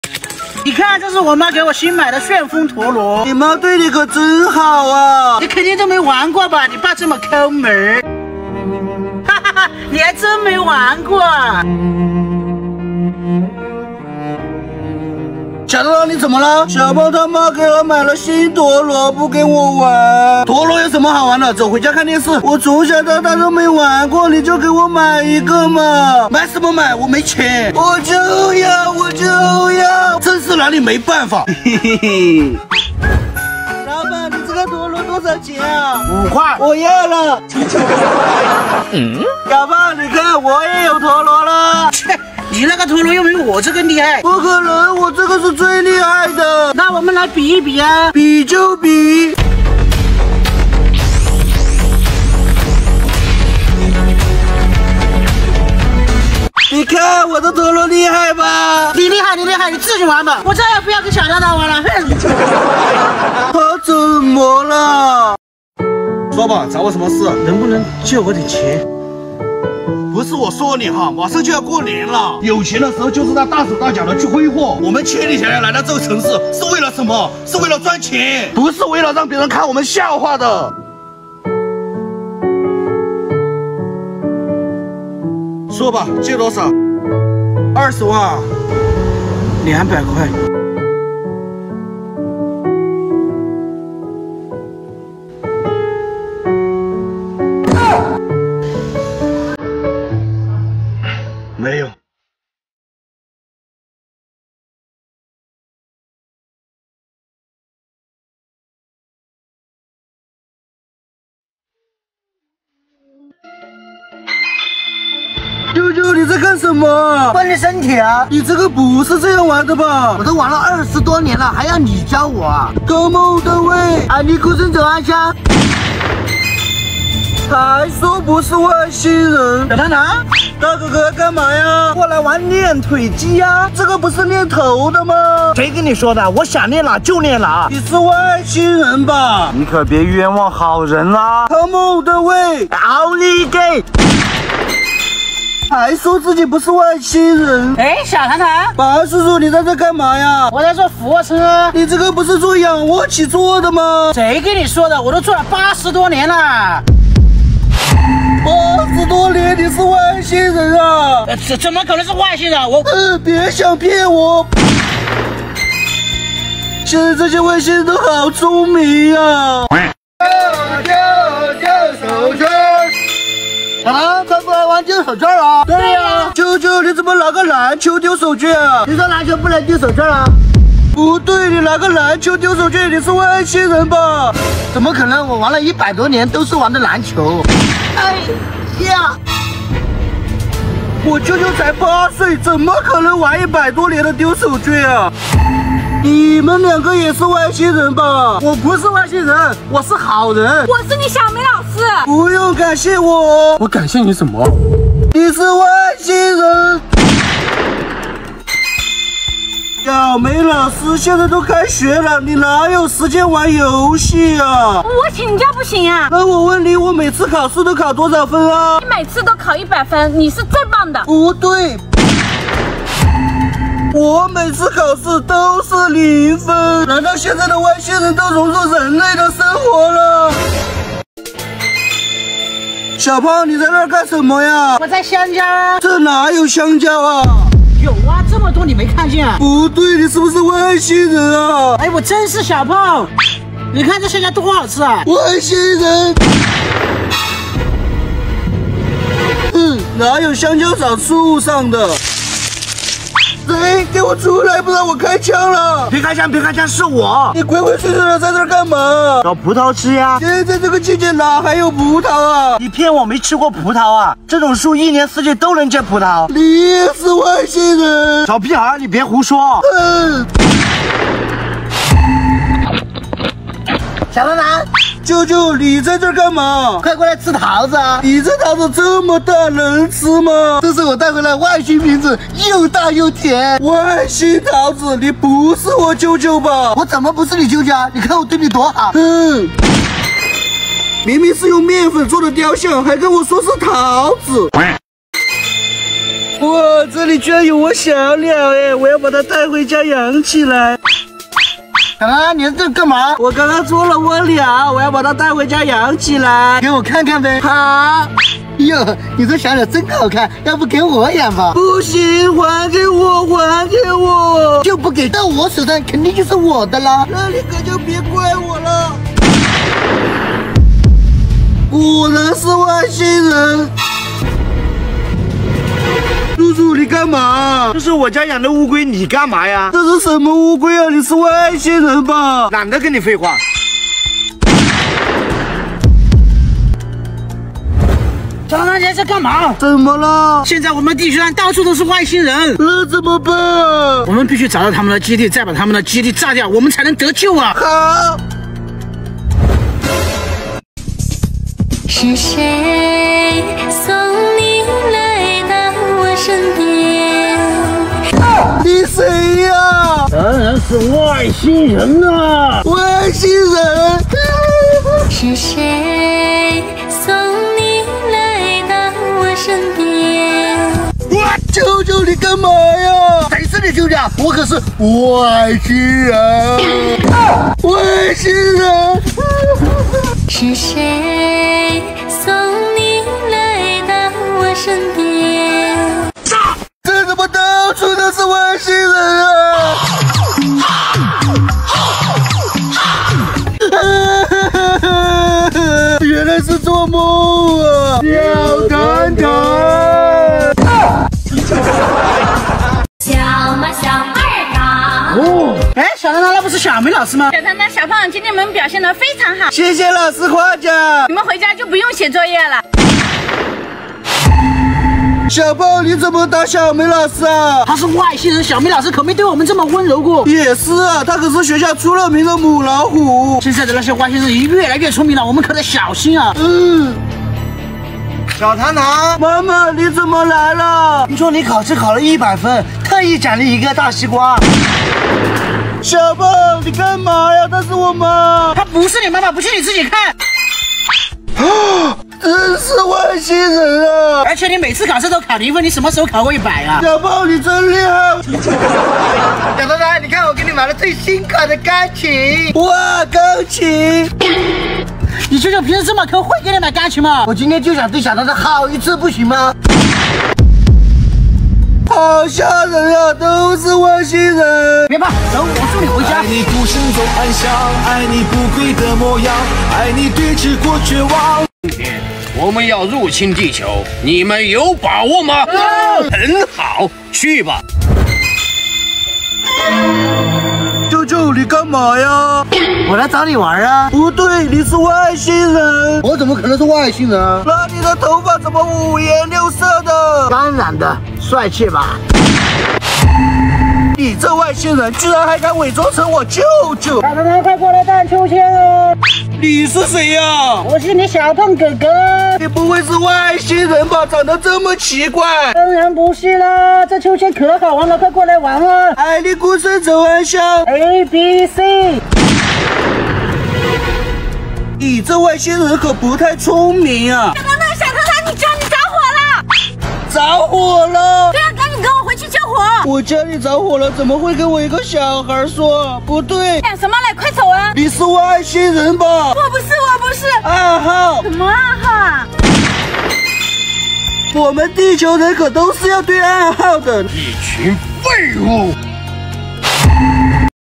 我從小到大都沒玩過，小胖為什麼不跟我玩了？就因為我的陀螺比他的厲害？
✦這裡是一小唐唐✦
在這裡分享每天的開心日常
有趣的靈魂萬裡挑一